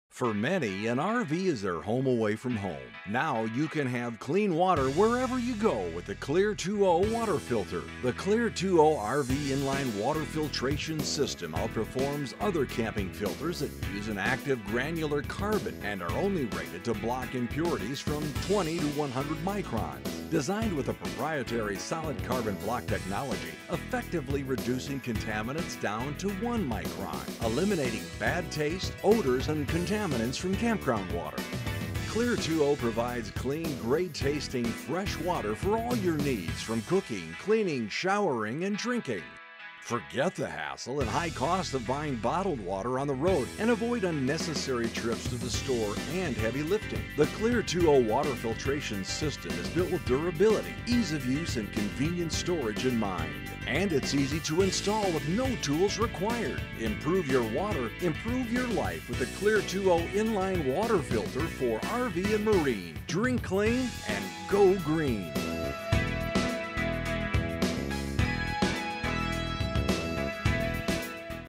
CLEAR2O RV & MARINE INLINE WATER FILTER CAMPGROUND
Our superior solid carbon block design filters your water down to 1 Micron reducing contaminants, Chlorine, bad taste and odors.  Your drinking water will now be crystal clear clean with no bad taste.  Easy to install.

Easy to Install.  No tools required.

Clear2O Go Clean and Go Green!

Walmart and Amazon.